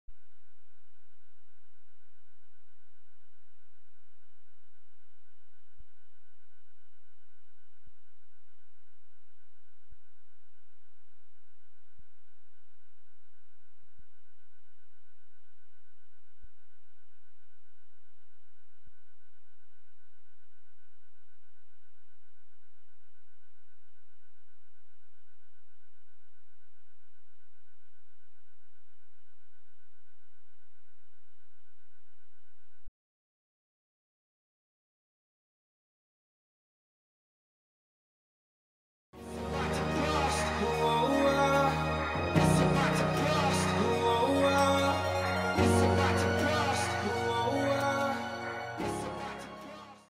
Throwback Thursday: Haseeb Hameed hits two centuries against Yorkshire
Throwback Thursday: Lancashire batsman Haseeb Hameed made history this year as the nineteen year-old hit two centuries against Yorkshire in the County Championship at Emirates Old Trafford.